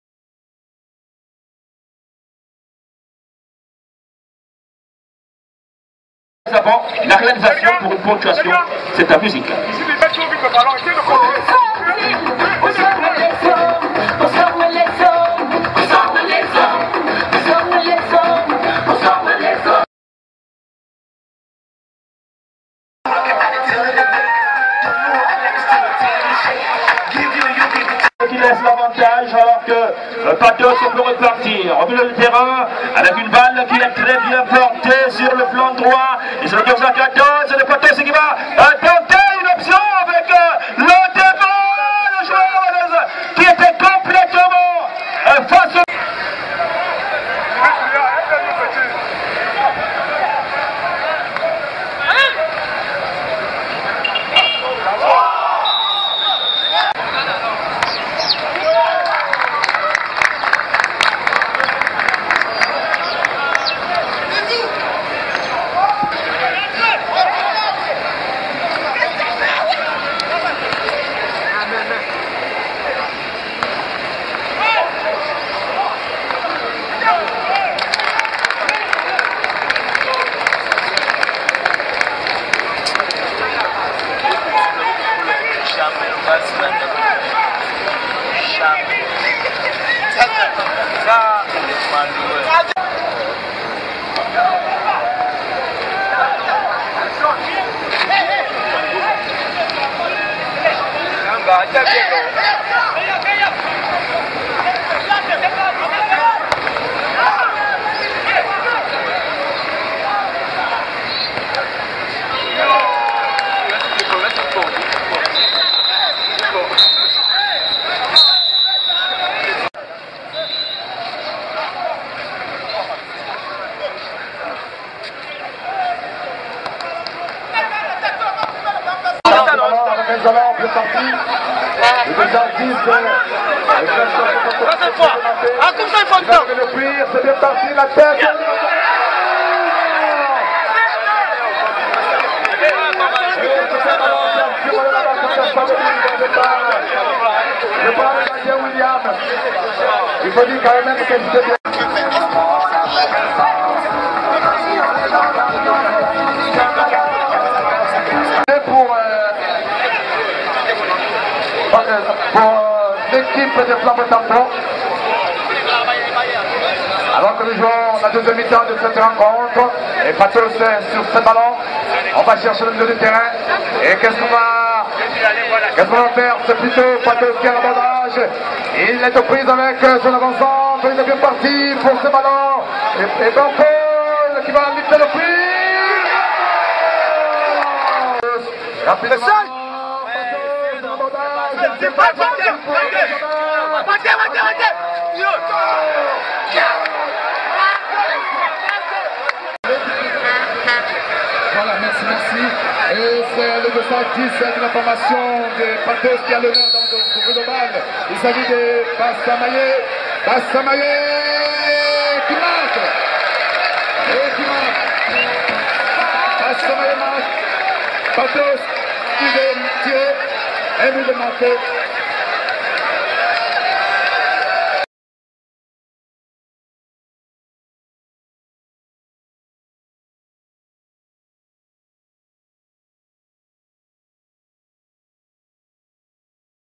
DIDIER BASSAMAGNE,international cameroonian striker
BORN in 1992, with 1.88m,he is  physically and technically fit.good organisation of the game and good vision ,he is very good scorer with his powerful shoot and heading.